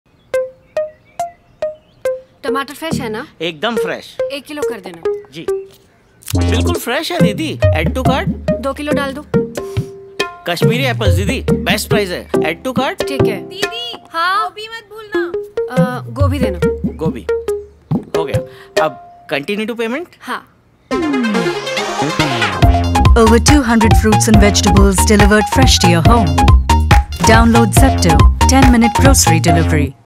Zepto - 10 Minute Grocery Delivery | Fresh Fruits & Vegetables
Over 200 fresh fruits and vegetables delivered to the comfort of your home in 10 minutes! Download the Zepto app today!

Film Credits:

Agency: L&K Saatchi & Saatchi
Jt.NCD: Kartik Smetacek
Chief Strategy Officer: Snehasis Bose
Executive Vice President: Debarjyo Nandi
Strategic Planning: Shifra Baviskar
Creative Team: Rishabh Dave, Divakar Shetty
Account Management: Noor Quereshi, Ajay Pattem
Production House: Content Factory
Director- Amit Satyaveer Singh
Senior Producer- Meesam Raza
Content producer - Siddharth Singh
Dop - Varun Sud